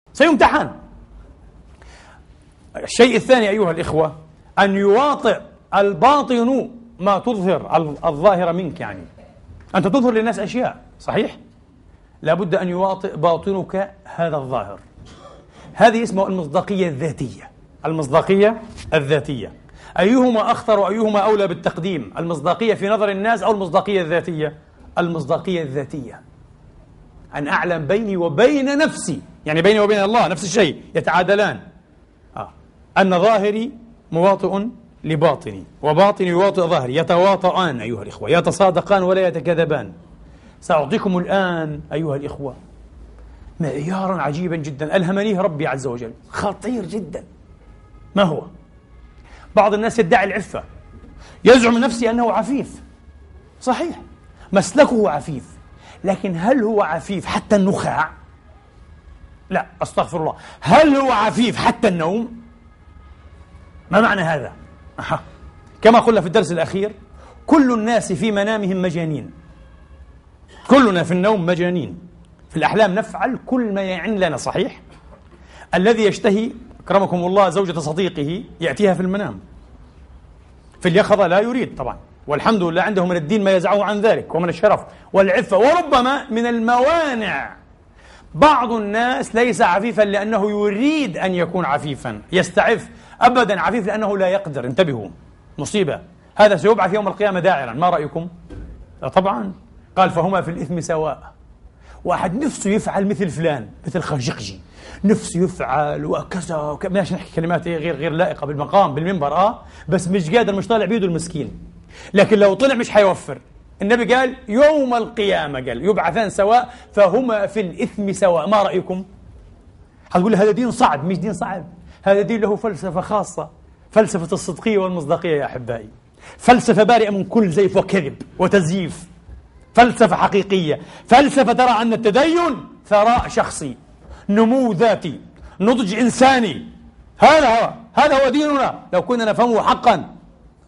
المصداقية الذاتية - عدنان ابراهيم │ Self Credibility - Adnan Ibrahim
في سياق الحديث عن ماهية المصداقية من خطبة الدكتور عدنان ابراهيم "أين المصداقية؟"